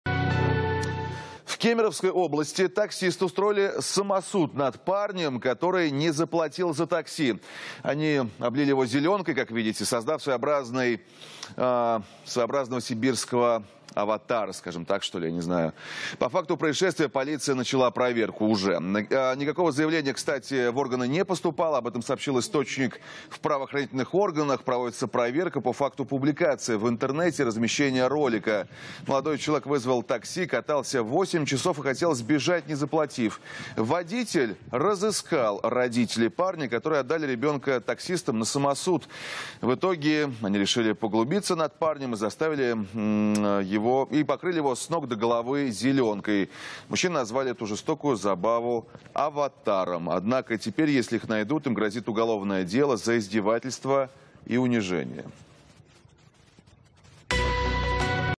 Полиция ищет таксистов, которые издевались над клиентом
Таксисты облили не оплатившего проезд клиента зеленкой